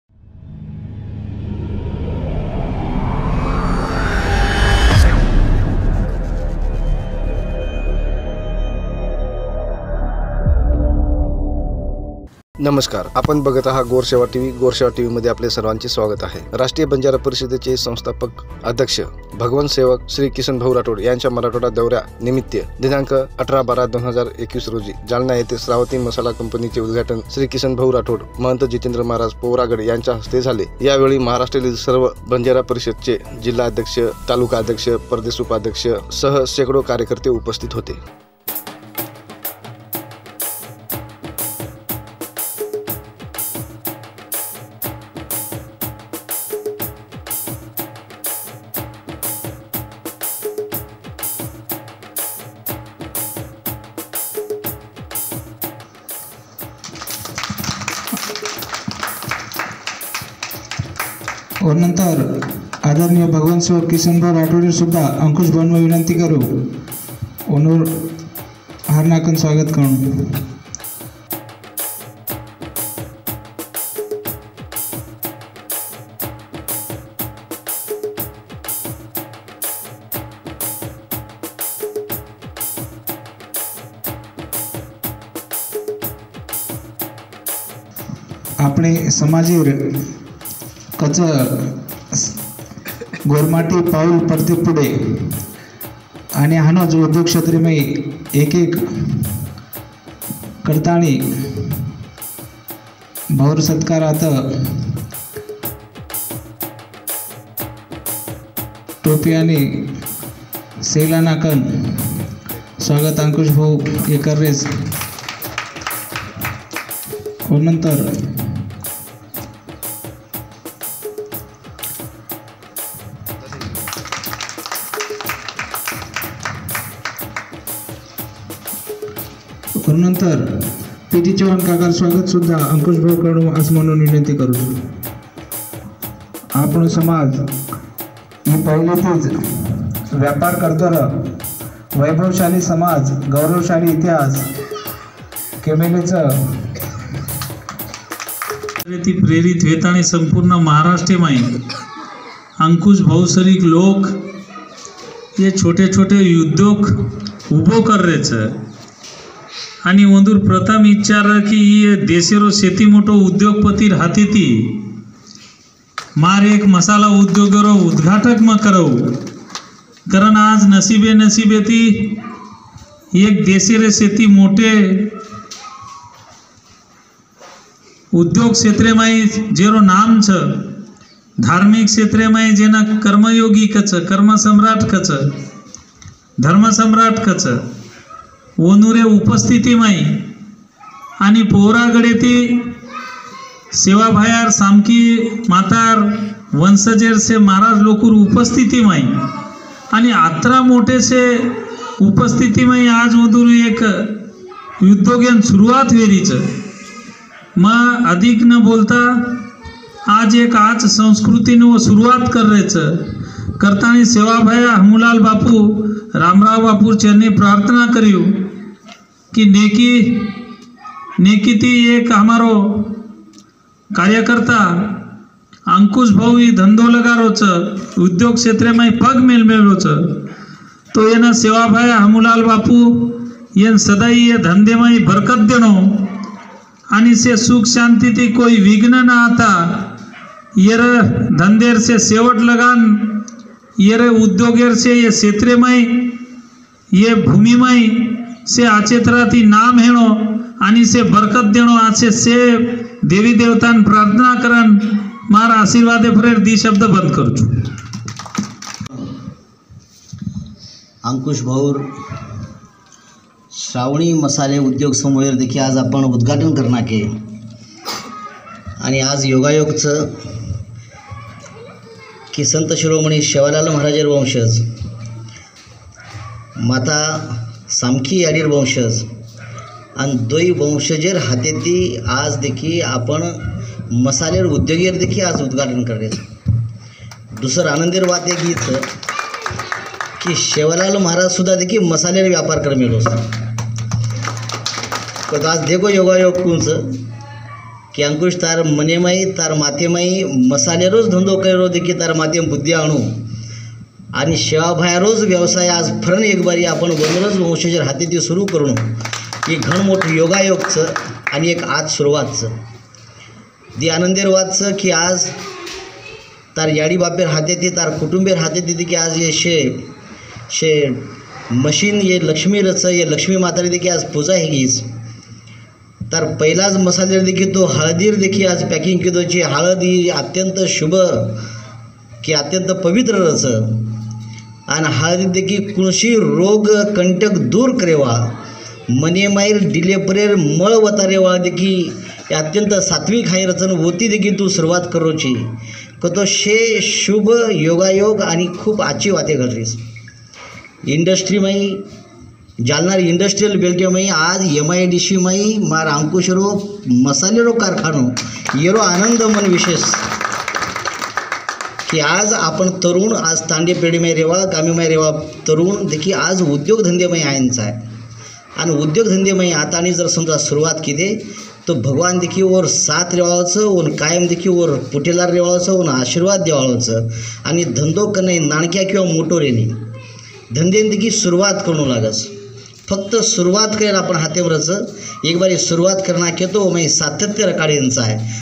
क्षावनी मसाला कंपनी उदघाटन भगवंत सेवक Kisan Bhau Rathod यांच्या हस्ते जालना येथे संपन्न झाले.
GOR SEVA TV

RECORDING STUDIO
SEVA RECORDING STUDIO
KARLA ROAD, PUSAD DIST- YAVATMAL